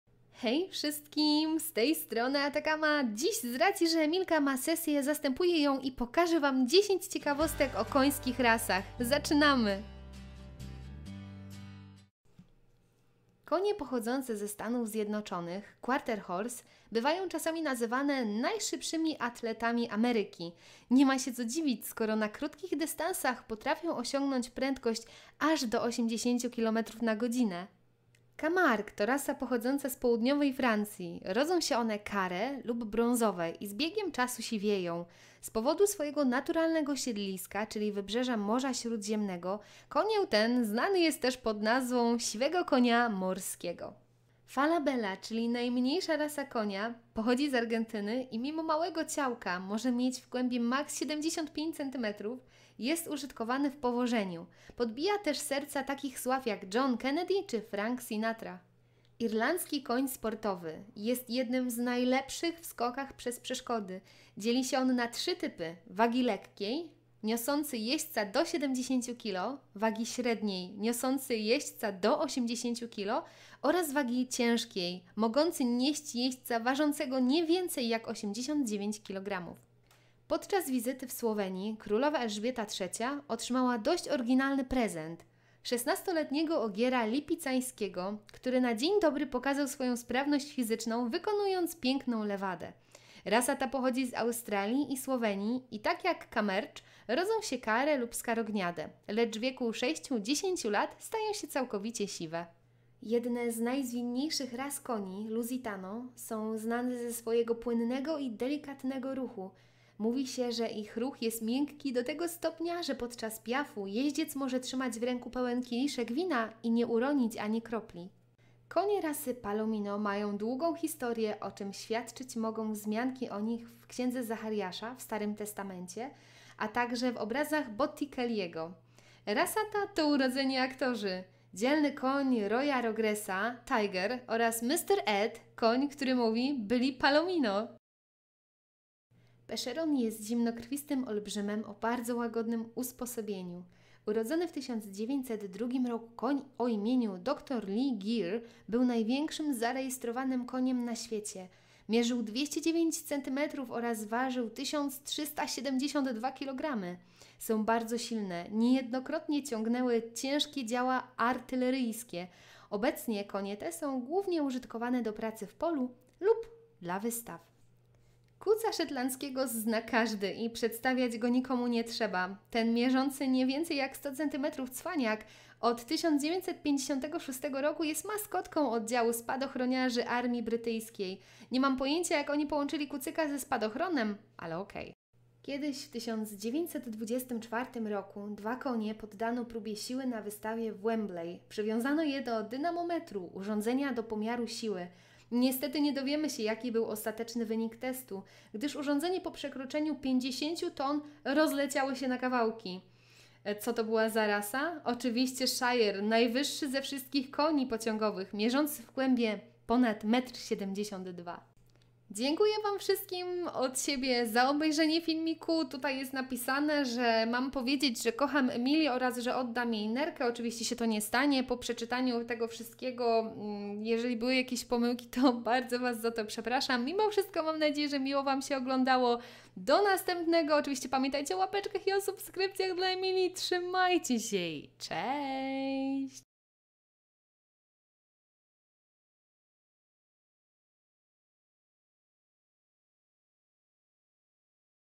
10 ciekawostek o końskich rasach /by ATTACCAMA
↓↓ Rozwiń opis ↓↓

Ciekawostki zaczerpnięte z książki ,,Konie", którą zakupiłam kiedyś w Biedronce :D
Zdjęcia z internetu :)